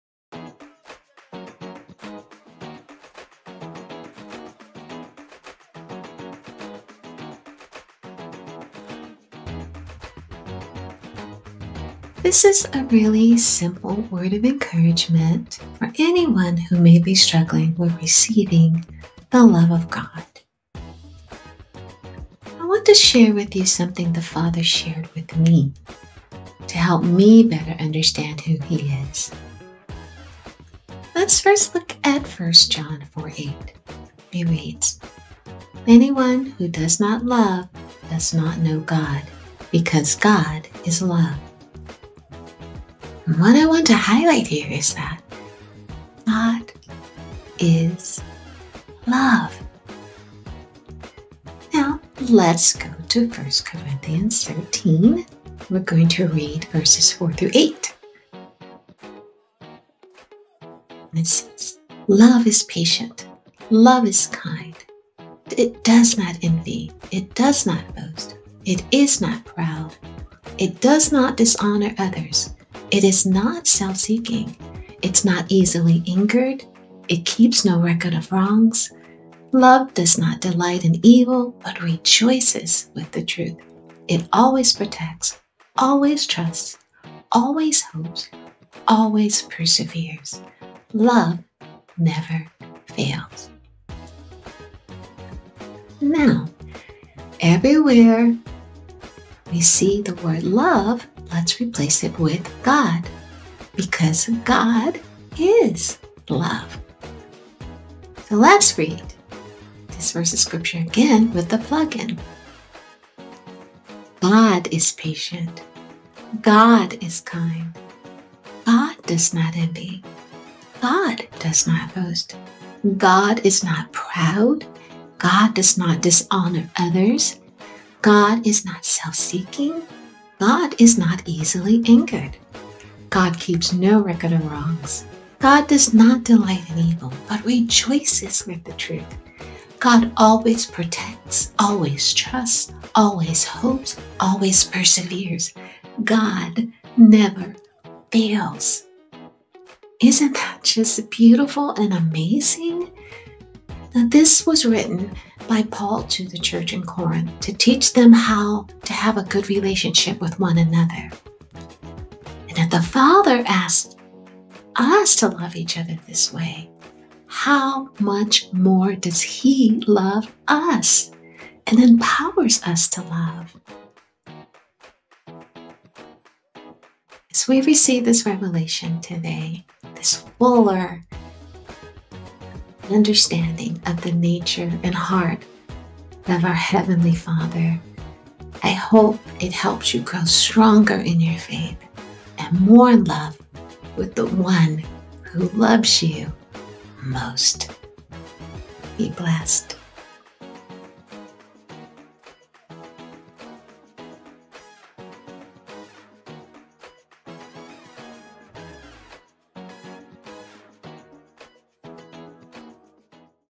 GOD IS LOVE
This is a very simple word of encouragement for anyone who may be struggling with understanding and receiving the love of God. I grew up with a dad who was a strict disciplinarian, and so when it came to my relationship with the Father, I struggled somewhat in receiving His mercy and grace. I still felt as if I had to gain His “unmerited” favor… His grace.

I know there is a time in our faith-walk where we do have fruit in our lives that prove our love for the Father, however, some cannot even get through the door of knowing Him because they are met at the threshold with a requirement of being “good enough”. We have to give people the opportunity to go through the door, and then allow the Holy Spirit to begin the process of conforming us to the image of Christ. Be blessed!